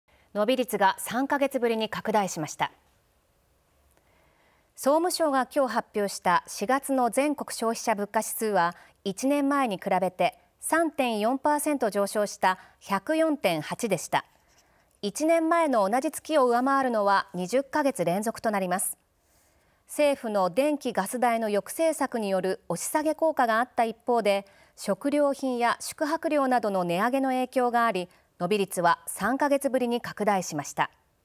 ４月消費者物価3.4％上昇（2023年5月19日）
伸び率が３カ月ぶりに拡大しました。総務省がきょう発表した４月の全国消費者物価指数は１年前に比べて、3.4％上昇した104.8でした。１年前の同じ月を上回るのは20カ月連続となります。政府の電気・ガス代の抑制策による押し下げ効果があった一方で、食料品や宿泊料などの値上げの影響があり、伸び率は３カ月ぶりに拡大しました。